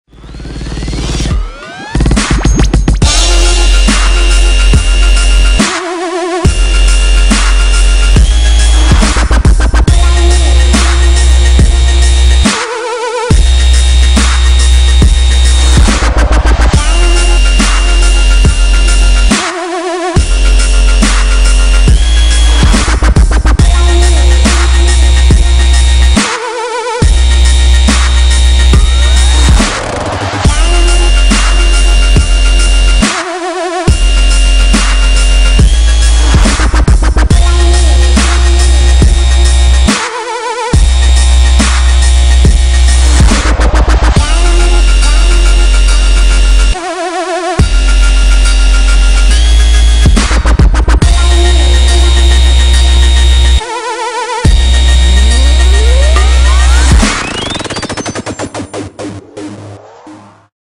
VEWLIX DIAMOND CUSTOM ARCADE FIGHT STICK XBOX 360 PS3 PC
Here is another clear case I built using an original Vewlix Diamond control panel (supplied by customer) cut down to a more suitable size.

- Stock JLF stick and Sanwa buttons for main control panel
- Clear Seimitsu ps-14-kn's for the sides
- Paewang/Datel pcb
- Full custom solid clear acrylic case made up of 1" walls and 2" for the front (wrist resting area) for greater stability.
- Brushed stainless steel panel for underside with plexi over.

Having used a VLX without the stability board, I didn't like the way it used to almost see-saw on my lap so gave this version a better base area to prevent this.